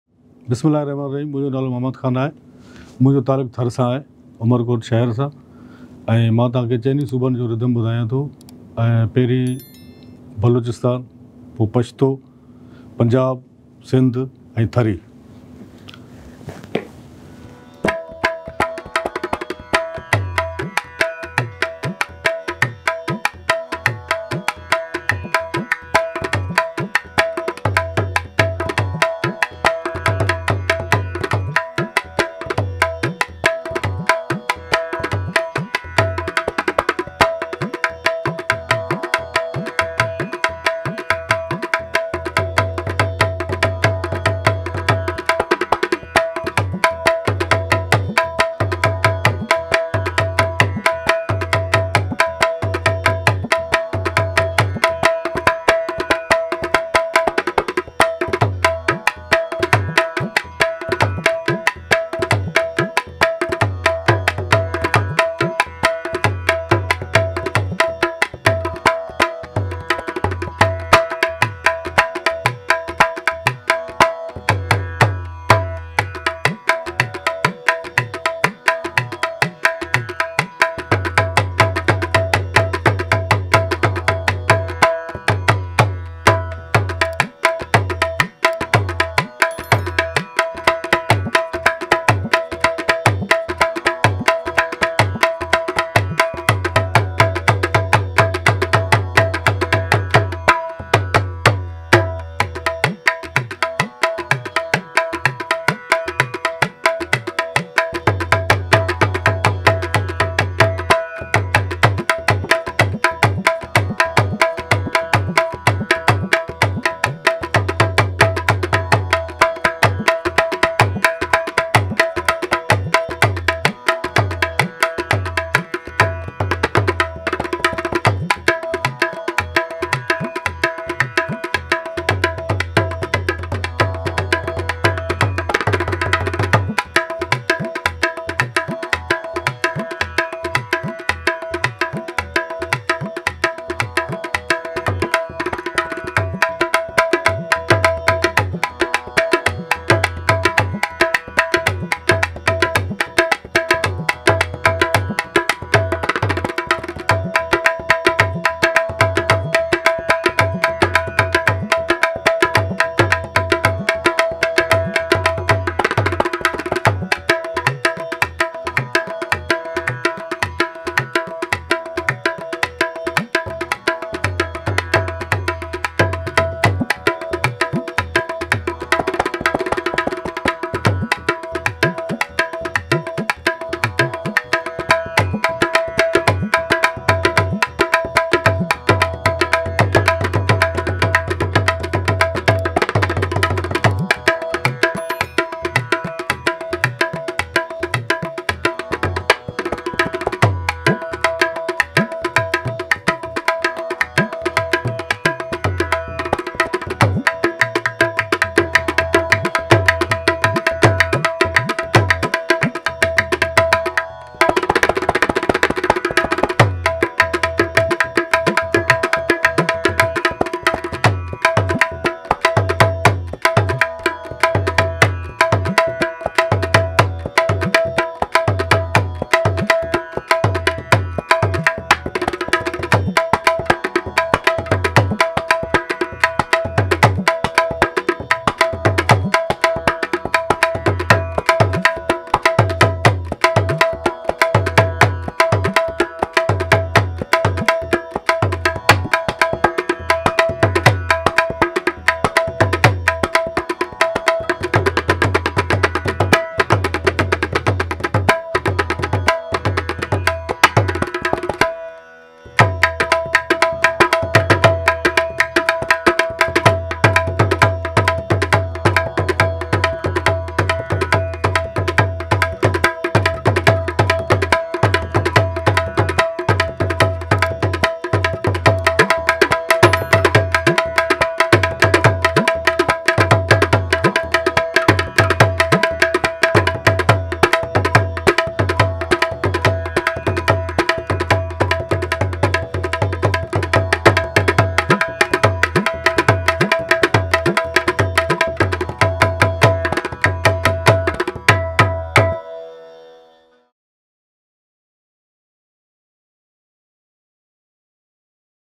Melodies of the 4 Provinces on Dholak by Nawaz Muhammad Khan | Baluchistan, KPK, Punjab, Sindh, Thar
Embark on a musical journey across Pakistan's diverse cultural landscape with the masterful Dholak Nawaz Muhammad Khan as he showcases the vibrant melodies of the nation's four provinces. From the rugged terrain of Baluchistan to the lush valleys of Khyber Pakhtunkhwa (KPK), the fertile plains of Punjab, the historic land of Sindh, and the enchanting desert vistas of Thar, each province's musical heritage comes alive through the rhythmic beats of the dholak.

In this captivating performance, Dholak Nawaz Muhammad Khan skillfully weaves together the distinct rhythms and melodies unique to each region, offering a glimpse into the rich tapestry of Pakistani culture. Whether you're a music enthusiast, a cultural aficionado, or simply curious about the diverse traditions that define Pakistan, this performance promises to delight and inspire.

Join us as we celebrate the cultural diversity and musical brilliance of Pakistan, uniting the nation through the universal language of music.